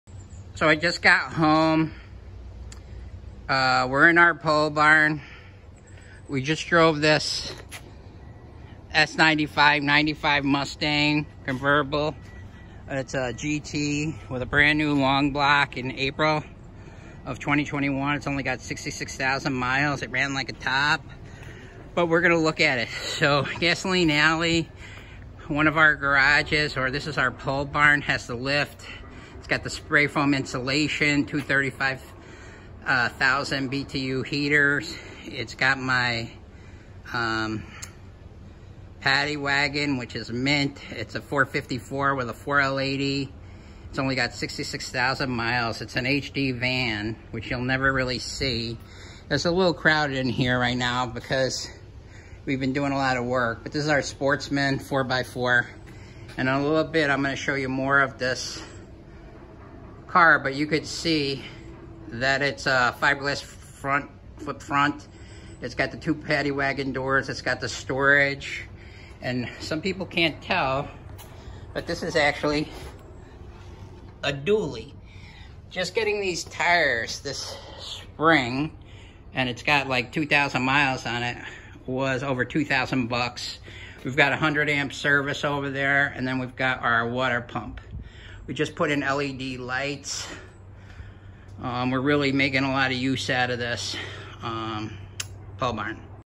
Gasoline Alley - 1995 Ford Mustang GT for sale, see inside our pole barn 1996 chevy HD30 van
1995 mustang gt convertible, 66,000 mi. new long block

rust free 1995 mustang gt with new 5.0 long block in April 2021, new ford performance clutch, new water pump, new paint